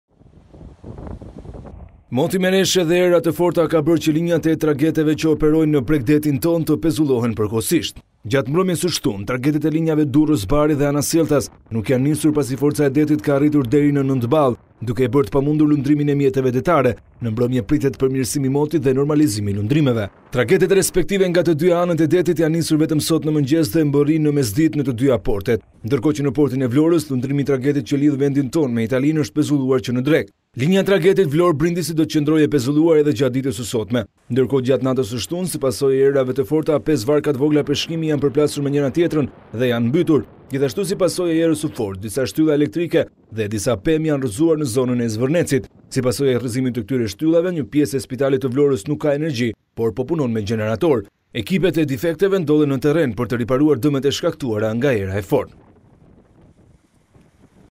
Report TV - Moti i keq, pezullohet lundrimi në Vlorë, fundosen 5 varka peshkimi
Mbi vendin tonë këtë të diel mbizotëron një mot me reshje lokale, ndërsa në terrenet malore janë në formë shtrëngate. Në vijën bregdetare frynë një erë e fortë, ndërsa në disa zona shoqërohet edhe me shi lokal. Kjo ka bërë që në qytetin bregdetar të Vlorës si pasojë e motit të keq, lundrimi të vijojë i pezulluar edhe ditën e sotme. Ndërkohë që raportohet se janë mbytur 5 varka të vogla peshkimi në zonën e Plazhit të Vjetër.

Tragetet e linjës Durrës-Bari dhe Bari-Durrës nuk u nisën mbrëmë për shkak të motit të keq, pasi forca e detit ka qenë 8 deri 9 ballë. Kjo ka bërë që tragetet të nisen sot në mëngjes dhe mbërrijnë në mesditë në të dyja portet. Trageti i nisur nga Ankona për Durrës do të mbërrijë sipas grafikut. Në mbrëmje pritet përmirësim i motit dhe normalizim në lundrimin detar.